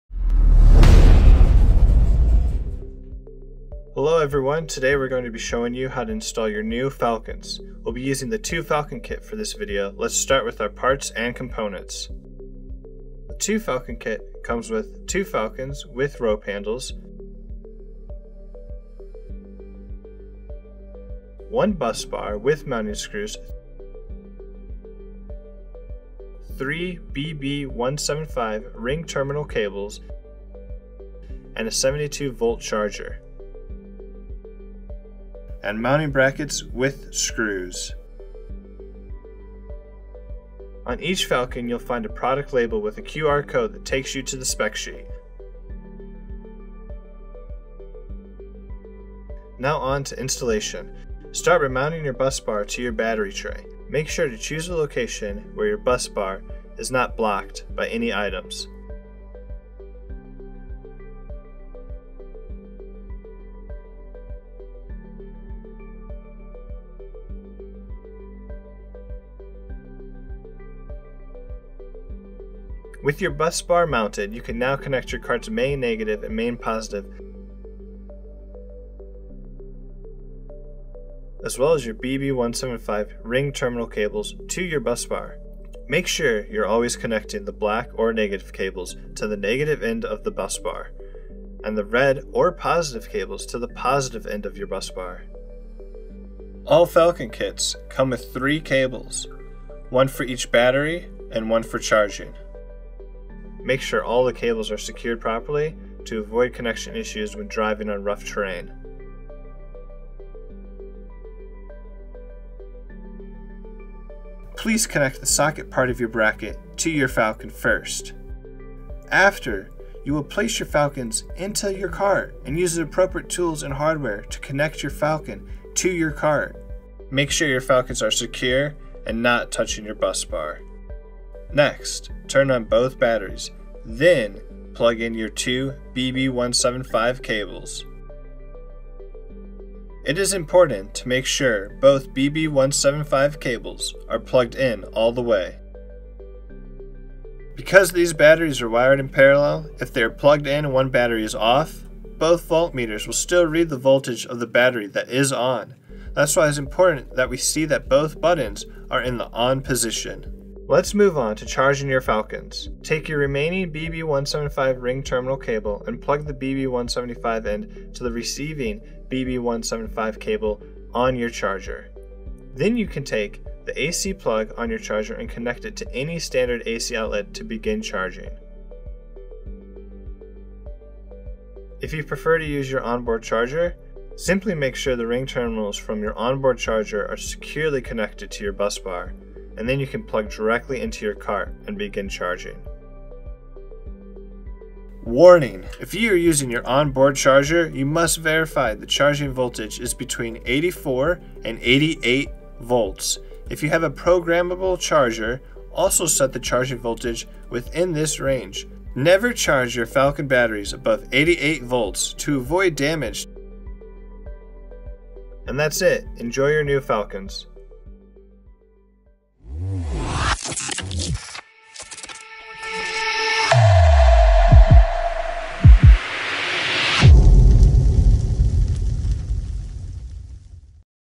72V FLCN | Golf Cart Installation
In this video we are going to learn how to install the new BigBattery 72V FLCN Kit into any compatible golf cart.